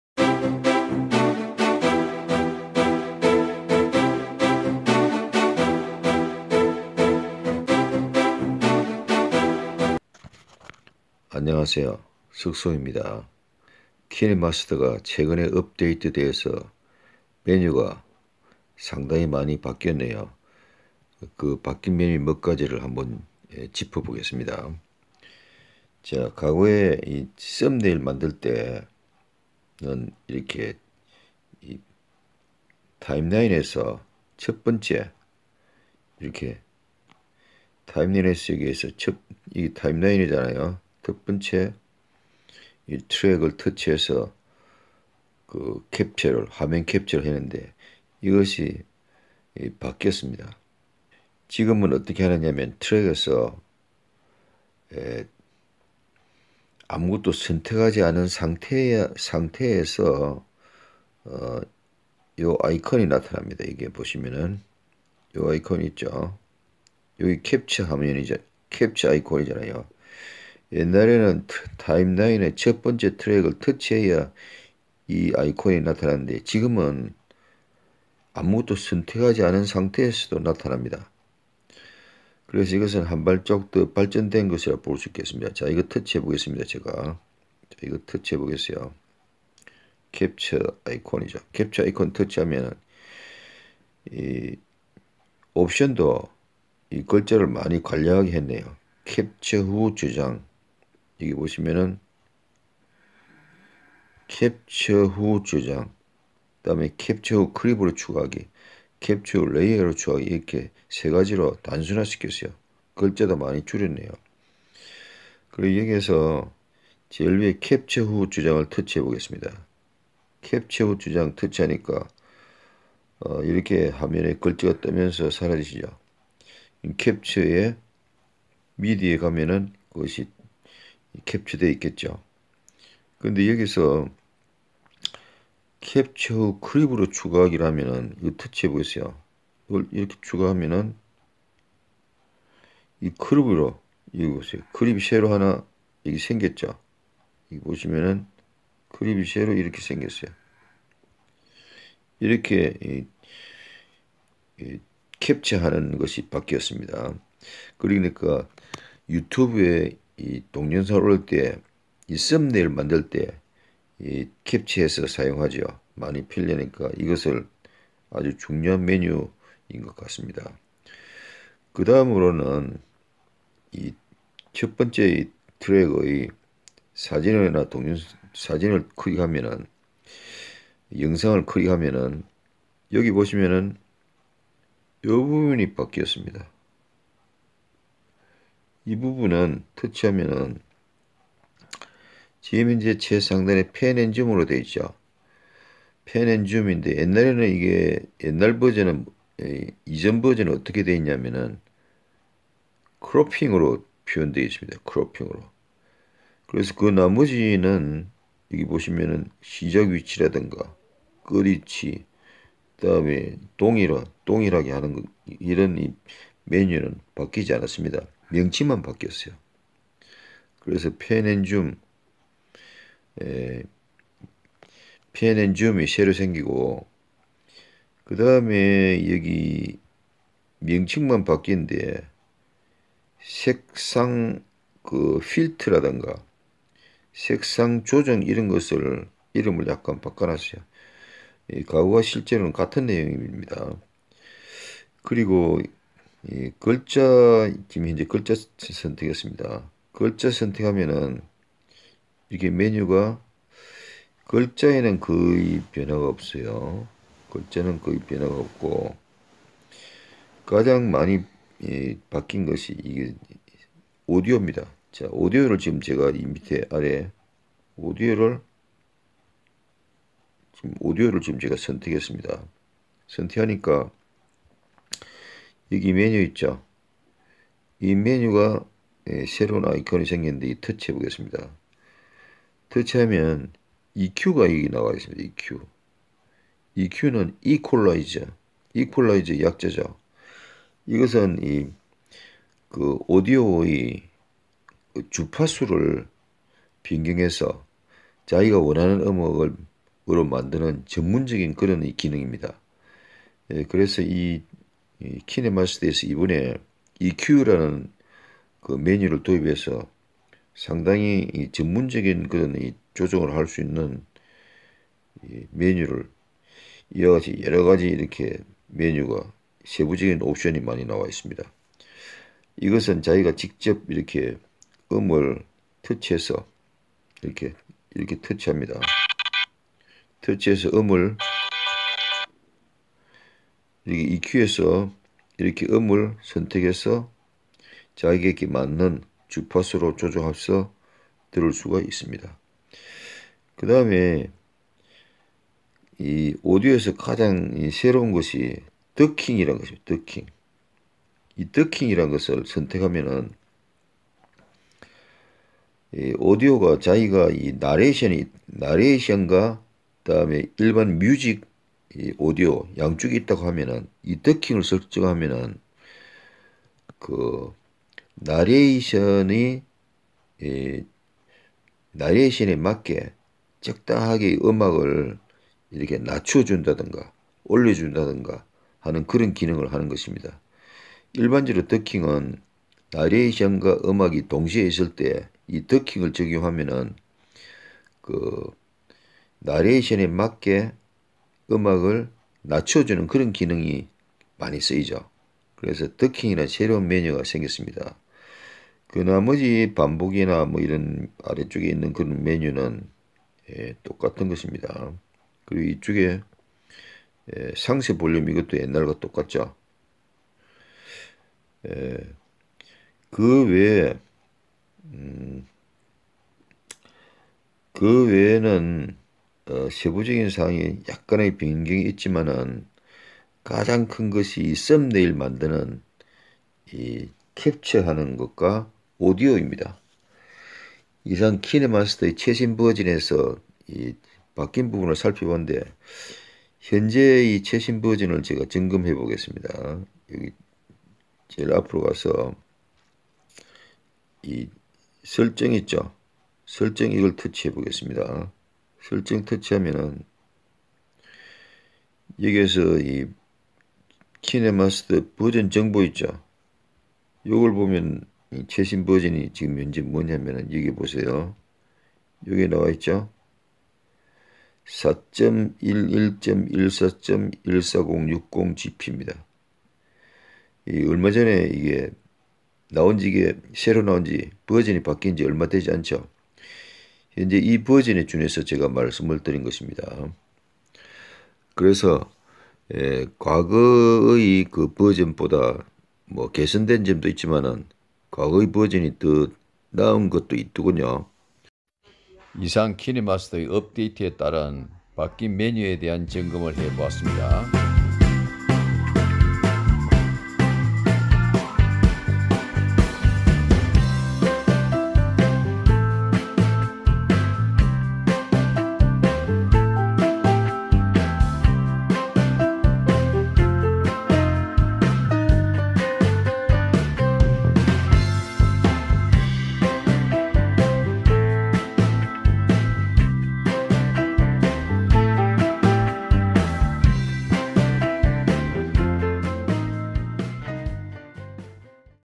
KineMaster 버전업으로 바뀐 메뉴 : 썸네일 캡쳐, 오디오 옵션 등
업데이트 키네마스터 에서 변경되 메뉴 캡쳐, 오디오 옵션 등